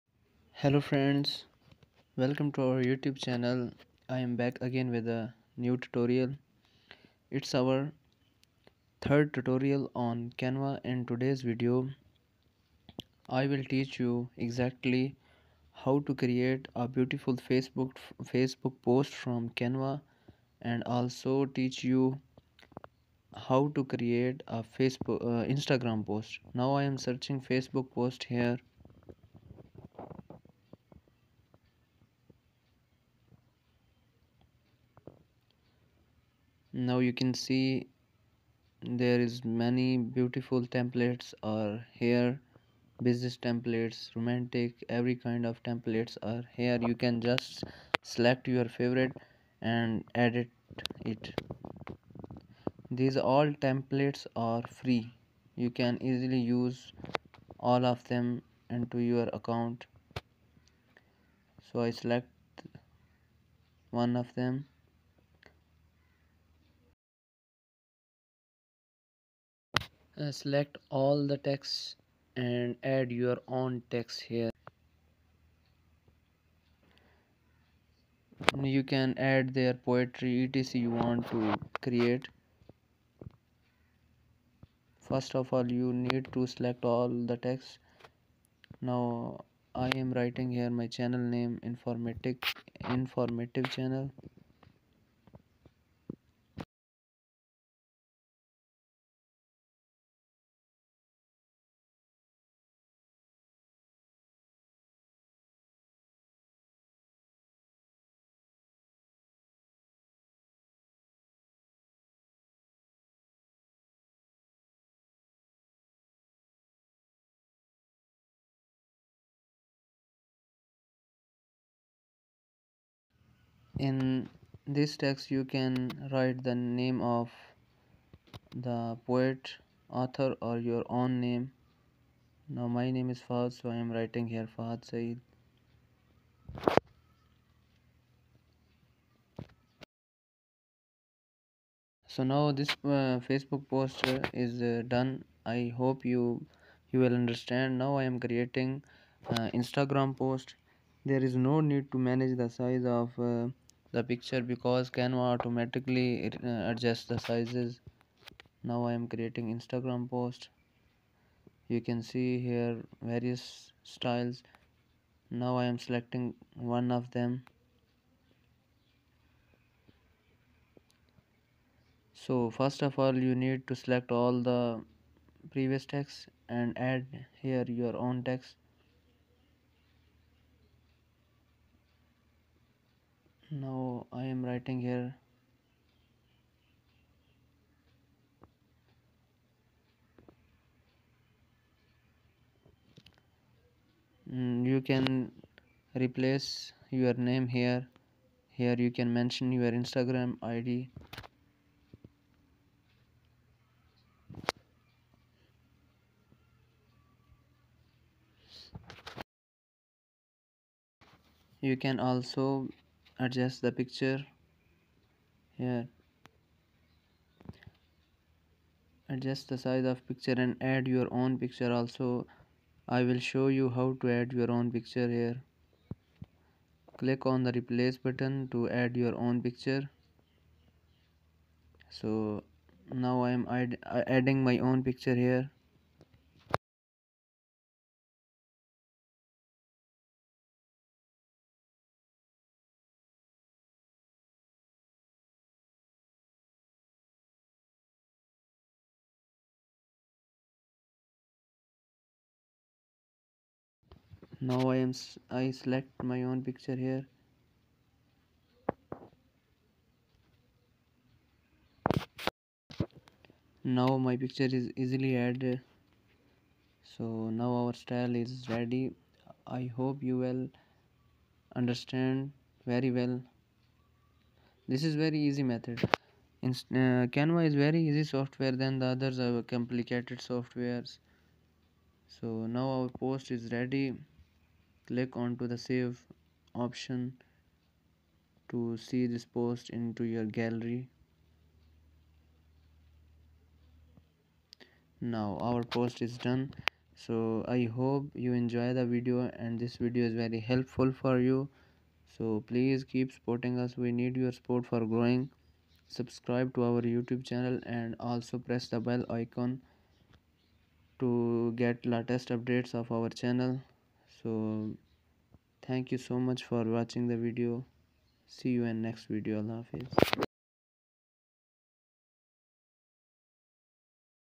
How to create Facebook /Instagram post from Canva
Hello Friends..

Welcome to Informative Channel.
A world that contains useful and helpful information, to shape, train, instruct, and educate you.

Its our 3rd tutorial on Canvas in this video I will teach you how to create Facebook/ Instagram post from Canvas. Thanks